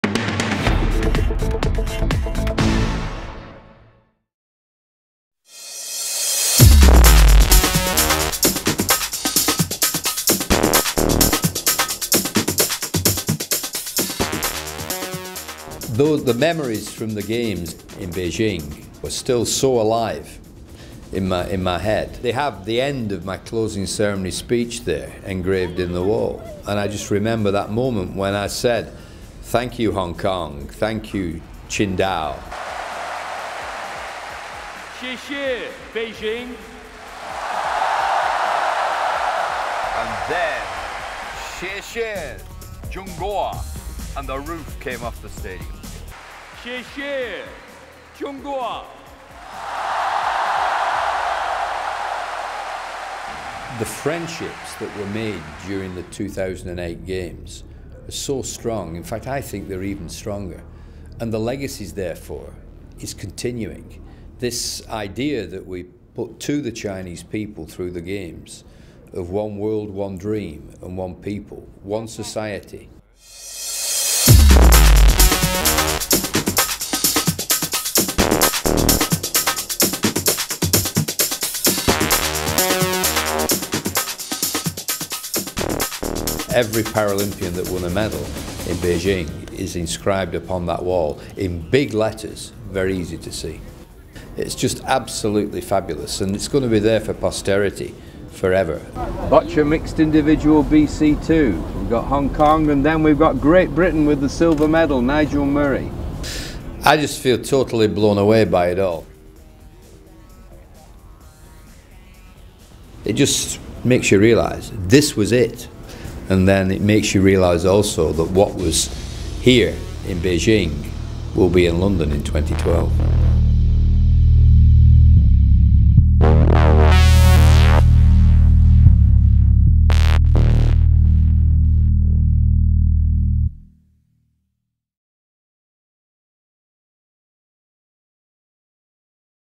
The legacy of Beijing continues to London
Sir Philip Craven, President of the International Paralympic Committee (IPC) reflects on the tremendous legacy of the Beijing 2008 Paralympic Games and how the momentum will continue in London and beyond.